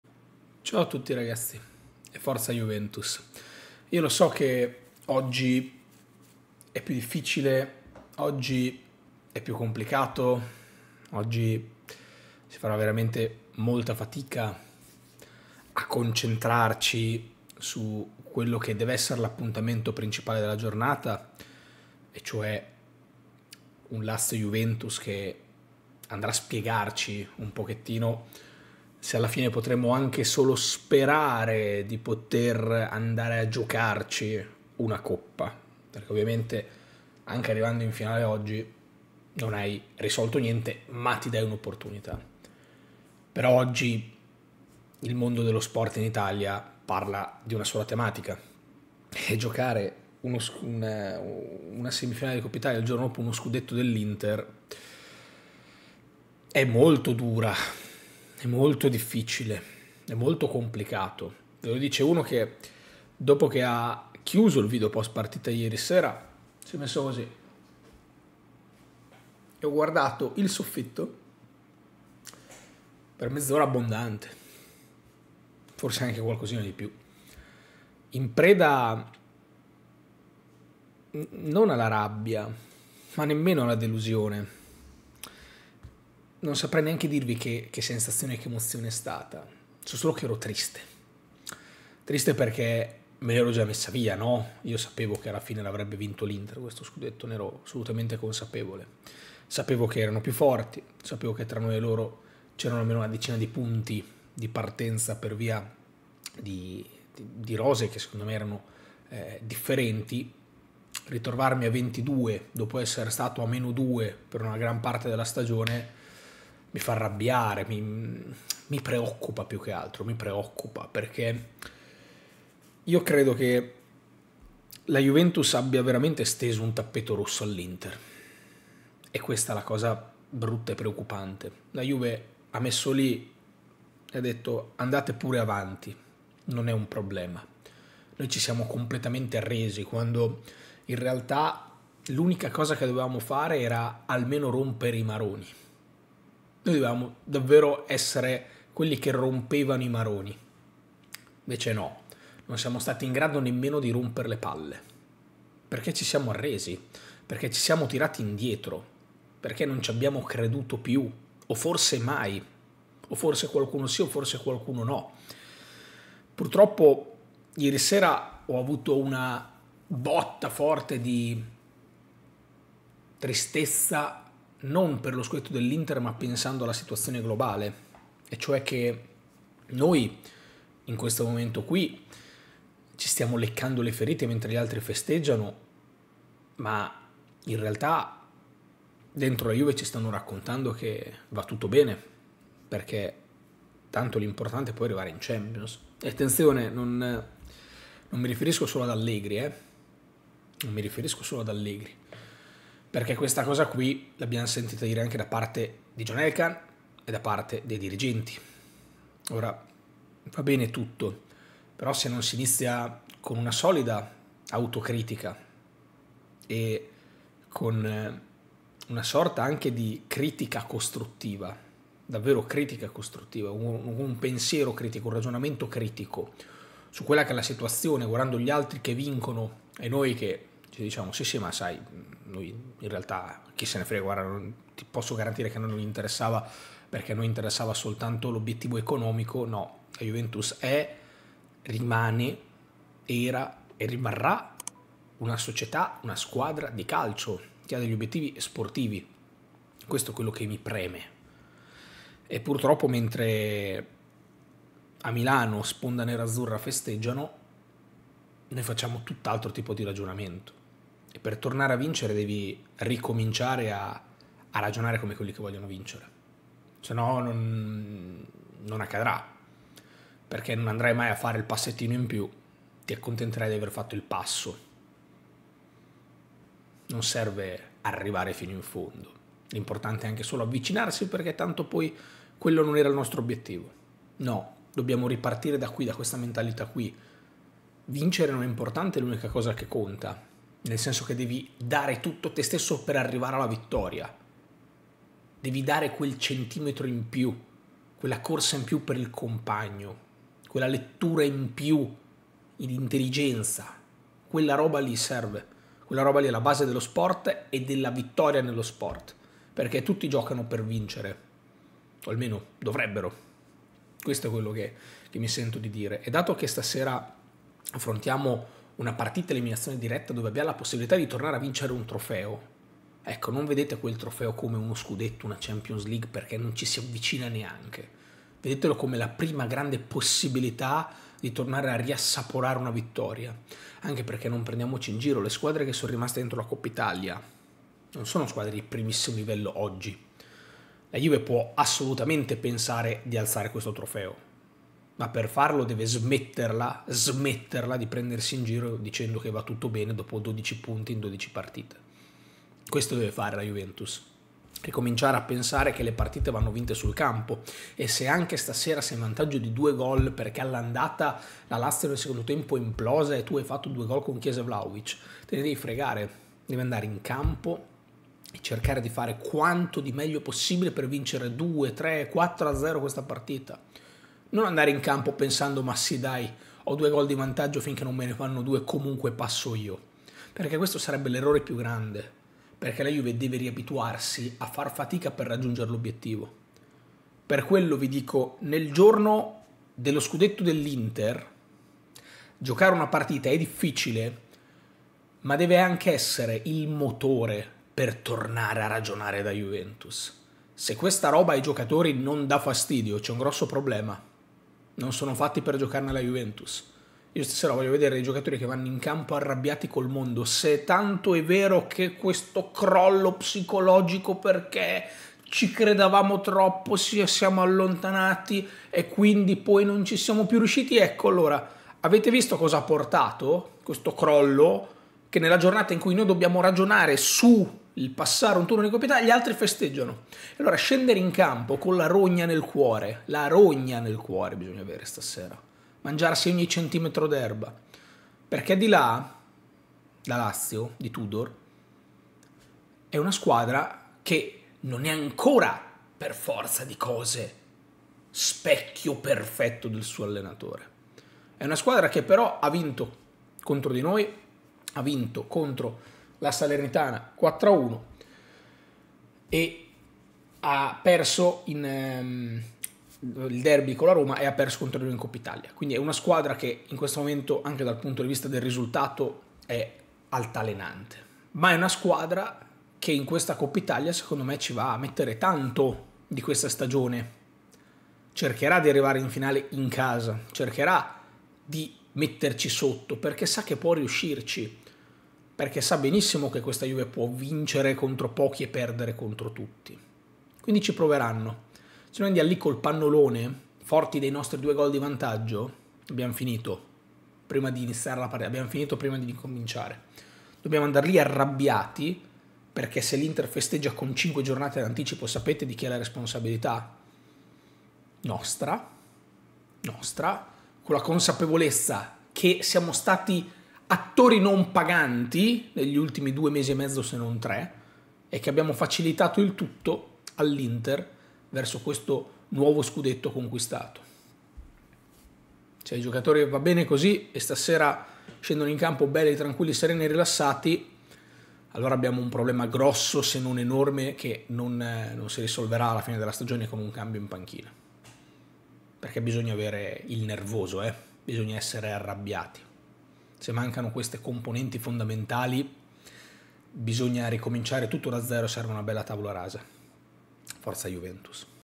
Sarà difficile giocare dopo uno Scudetto dell'Inter, ma... | Coppa Italia Lazio Juventus
Vietato pensare di essere già in finale soltanto per aver battuto Tudor all'andata.